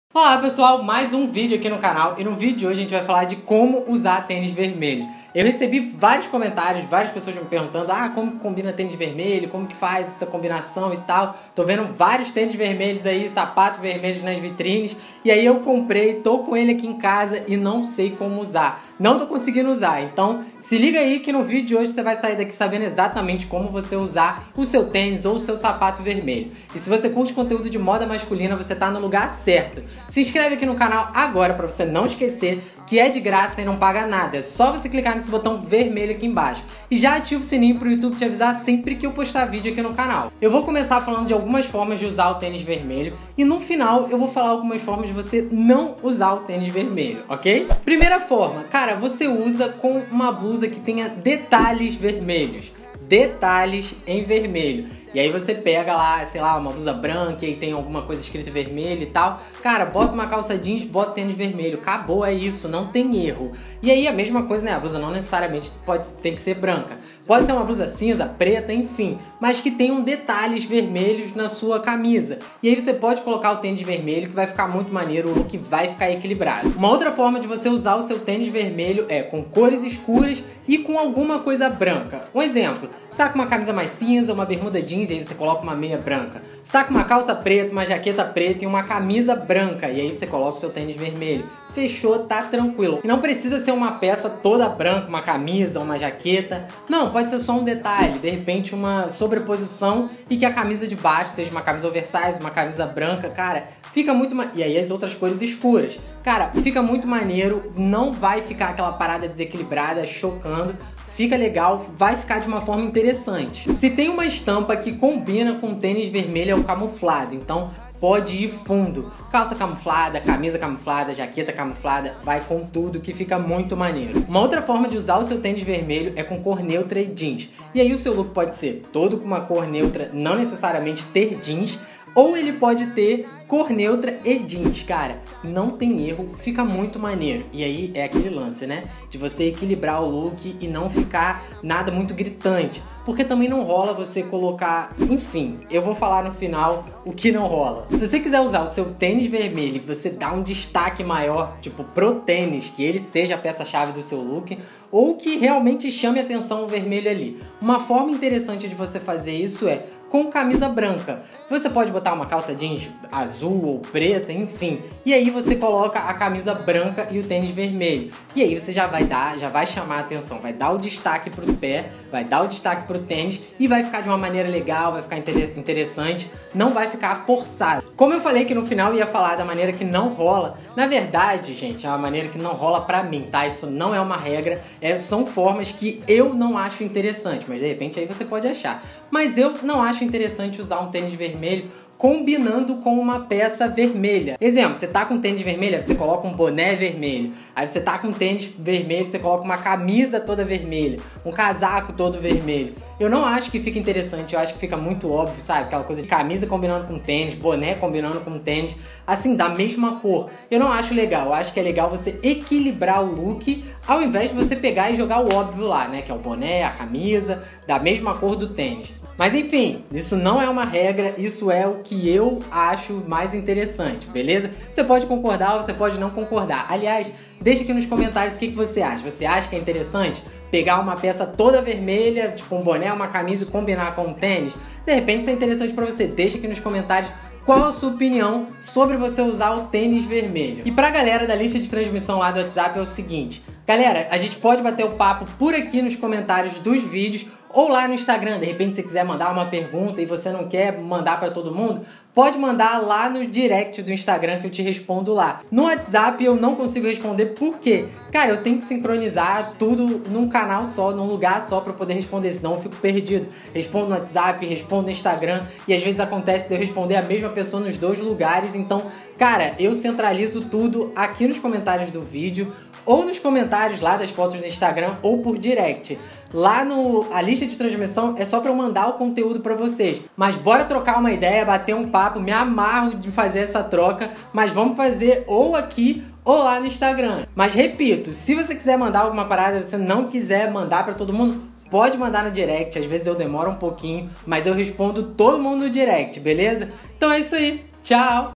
TÊNIS VERMELHO MASCULINO Como usar
TÊNIS VERMELHO MASCULINO Como usar. Alguns de vocês me mandaram mensagens pedindo dicas de como usar tênis vermelho e bordo. Então se liga nesse vídeo que você vai saber exatamente como usar, não só o tênis vermelho, como qualquer tênis colorido.